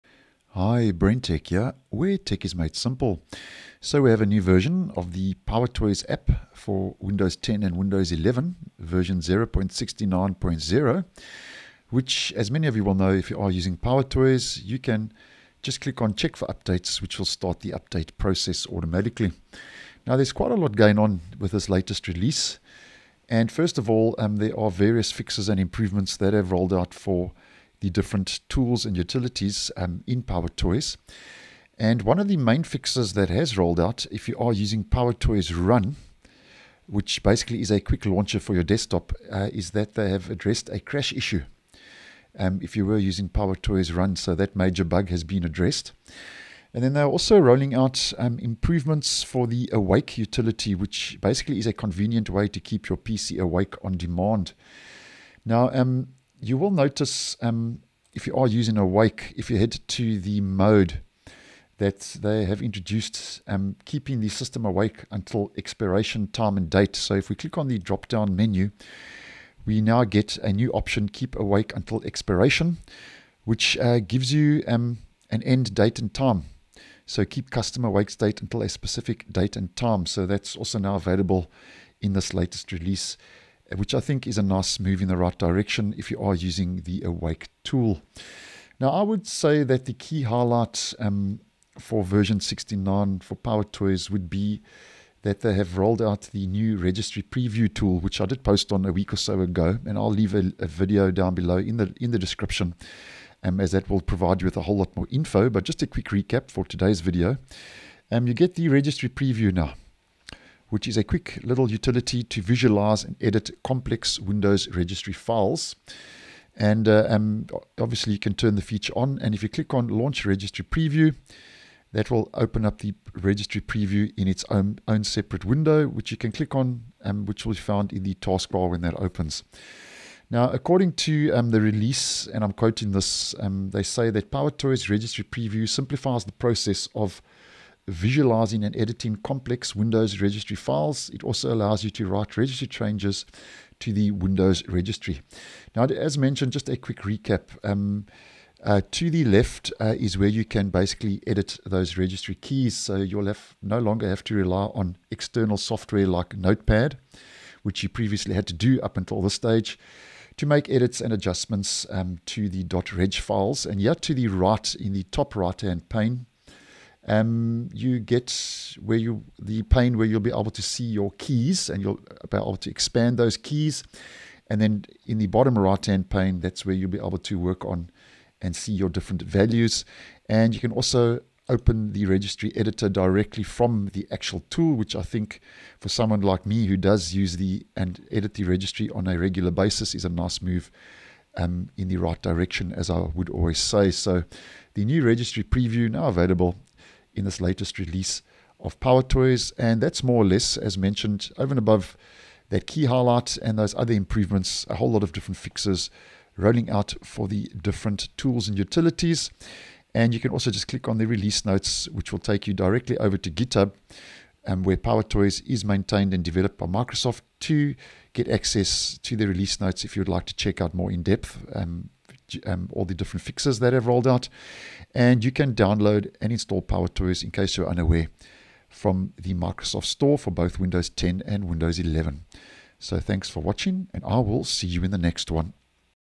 Microsoft PowerToys v0.69.0 released with new Registry Preview tool, bug fixes and more
This latest release includes various fixes and improvements, including fixing a crash issue in PowerToys Run and enhancements to the Awake utility. There is also a brand new utility, the new Registry Preview, that simplifies the process of visualizing and editing complex Windows Registry files. It also allows you to write registry changes to the Windows Registry.